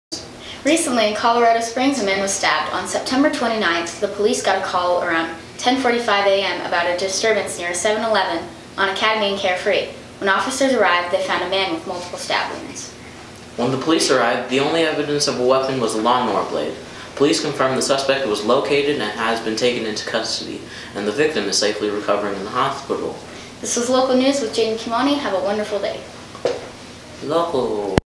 Local News: Lawnmower Blade Attack
The students researched, wrote, and edited a news story about local Colorado Springs news. This local news story is focused on an attack with a lawnmower blade. The design team included Kymani Williams and Jade Myers.